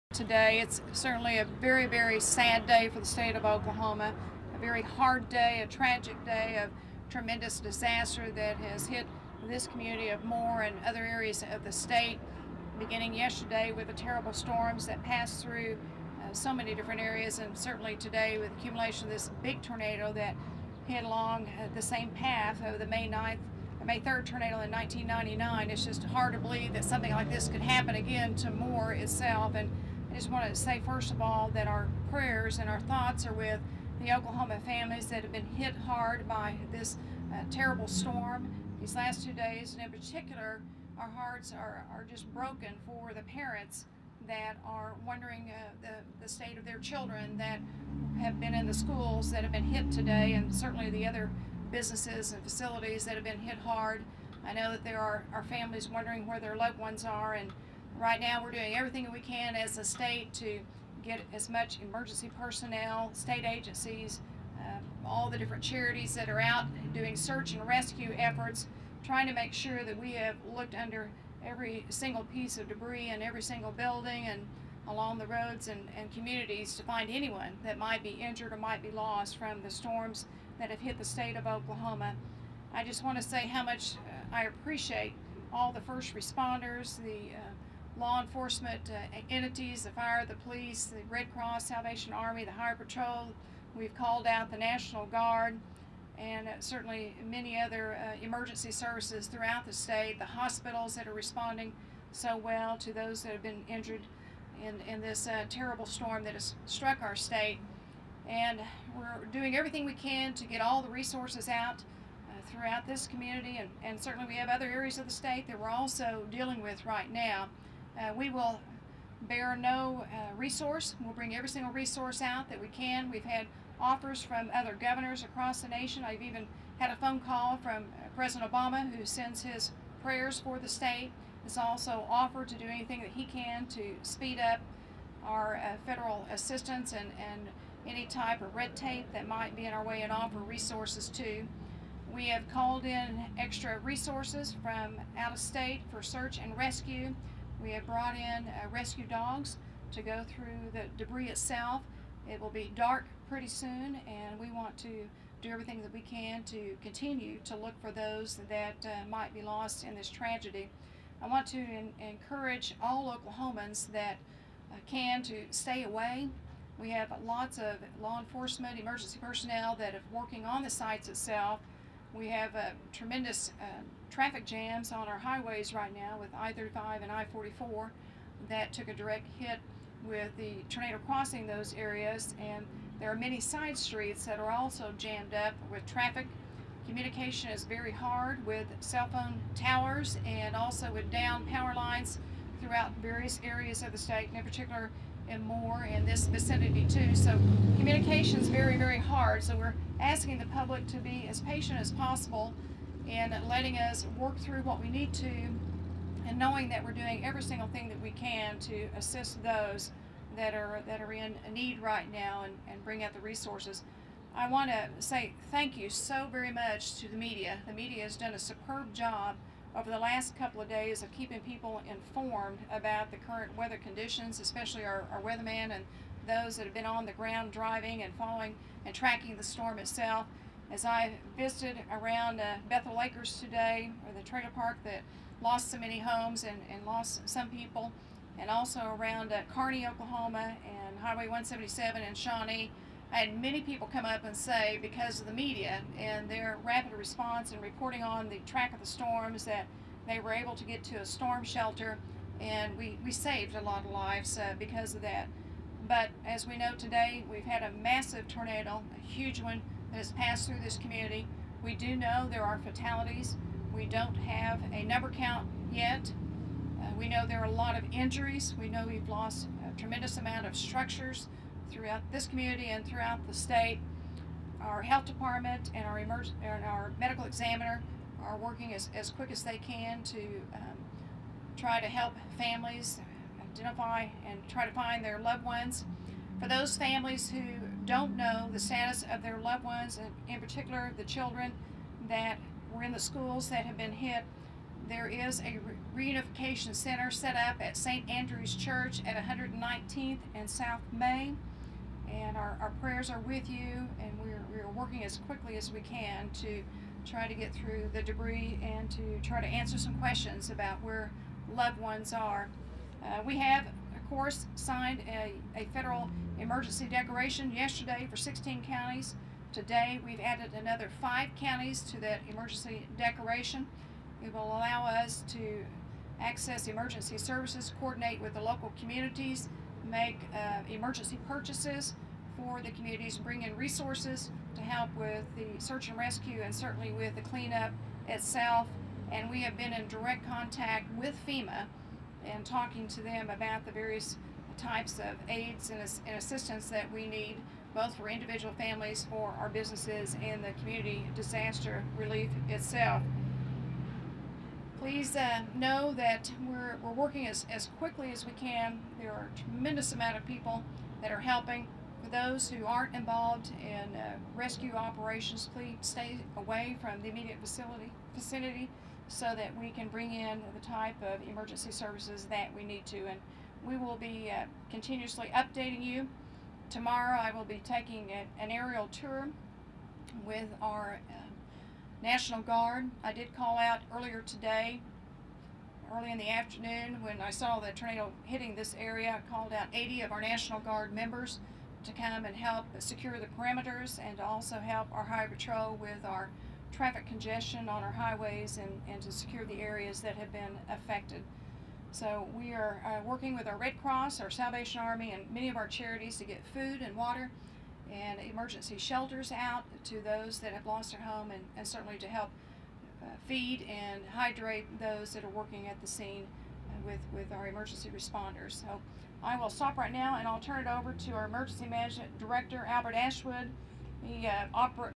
Oklahoma Governor Mary Fallin Talks About Moore, OK Tornado
Oklahoma Governor Mary Fallin spoke at a press conference about the Moore, Oklahoma tornado and the next steps to be taken in the aftermath.